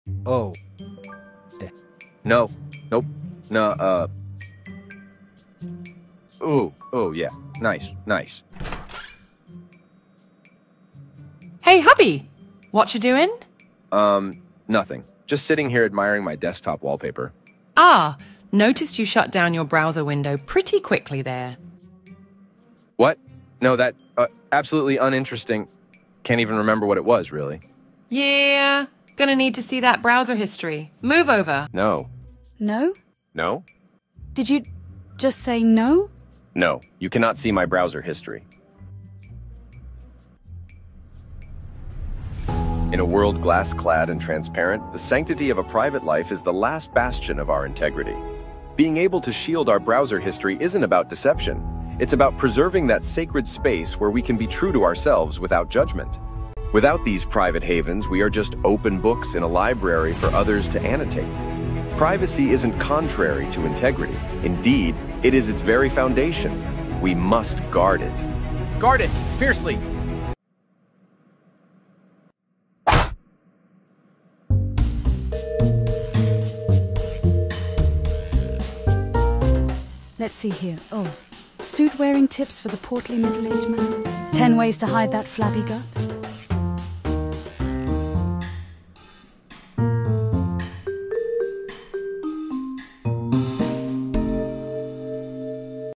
Hiding your browser history is a human right.
What a man looks at on the internet should be his affair, and his alone! Even if it is something... pretty horrible.